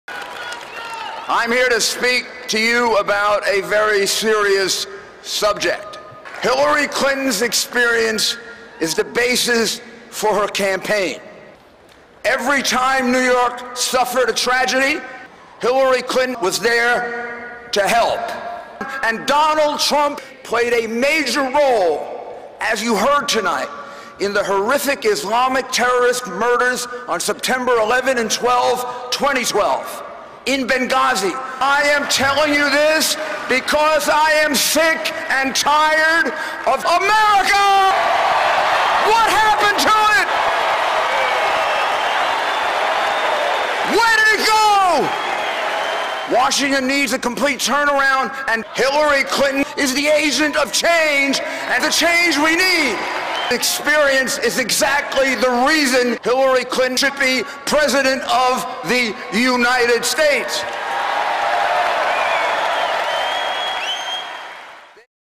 Giuliani Spills The Ugly Truth at 2016 RNC
A few things you might have missed from Rudy Giuliani at the RNC.